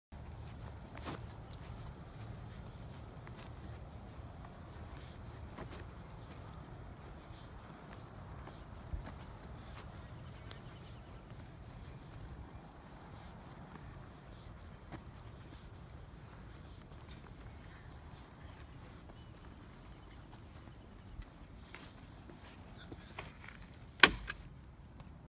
Sighting in my .22 rifle with scope at 100 yards
At first I was a little off on my shots but with some adjustments I was hitting the red of the target.